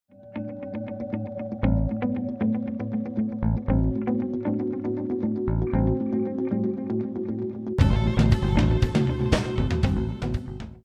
BACKROLL STRAPLESS
One of the trick of what you can learn at Suenosurf ProgressCamp.